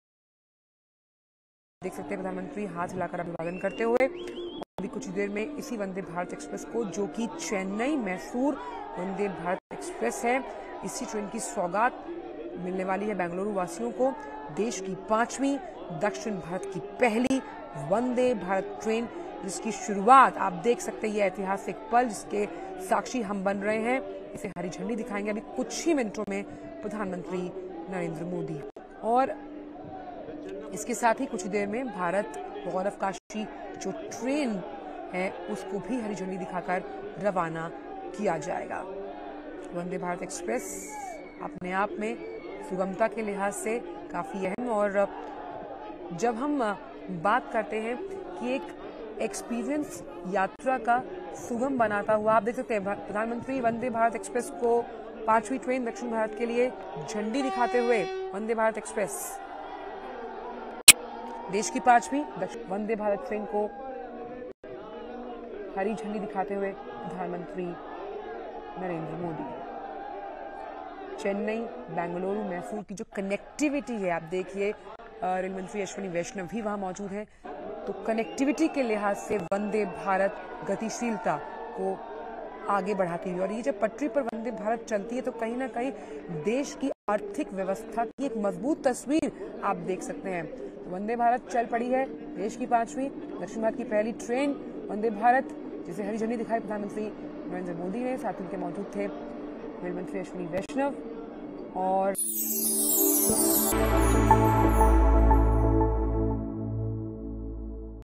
ವಂದೇ ಭಾರತ್ ಗೆ ಹಸಿರು ನಿಶಾನೆ | VandeBharatExpress | Lankesh App
ಕ್ರಾಂತಿವೀರ ಸಂಗೊಳ್ಳಿ ರಾಯಣ್ಣ ರೈಲು ನಿಲ್ದಾಣಕ್ಕೆ ಬಂದ ಪ್ರಧಾನಿ ಮೋದು ವಂದೇ ಭಾರತ್ ಎಕ್ಸ್‌ಪ್ರೆಸ್ ರೈಲಿಗೆ ಹಸಿರು ನಿಶಾನೆ ತೋರಿದ್ದಾರೆ. ಚೆನ್ನೈ ಕಡೆಗೆ ಸಂಚರಿಸುವ ರೈಲಿಗೆ ಹಸಿರು ನಿಶಾನೆ ತೋರಿಸಿದ್ದಾರೆ. ಬಳಿಕ ಪ್ಲಾಟ್‌ಫಾರ್ಮ್ 8 ರಿಂದ ಭಾರತ್ ಗೌರವ್ ಕಾಶಿ ದರ್ಶನ್ ರೈಲಿಗೆ ಚಾಲನೆ ನೀಡಿದ್ರು.